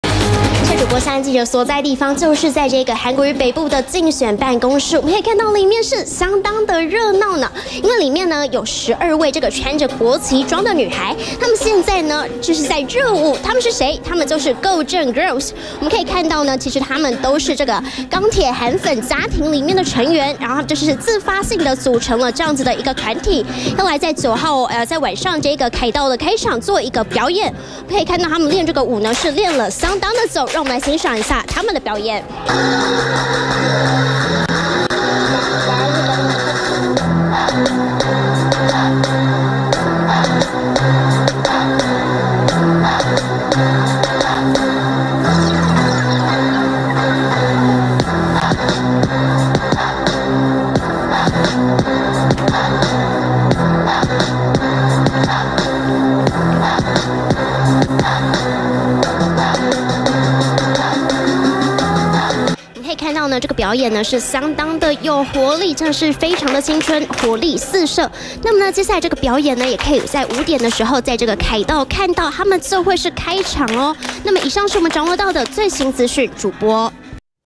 20200109中天新聞 韓號召庶民凱道造勢 Go政girls成「嬌」點
國民黨總統候選人韓國瑜，凱道造勢大會，今天登場，而其中一個焦點，就是由12位國旗女孩，組成的團體Go政girls，連線記者馮薇之。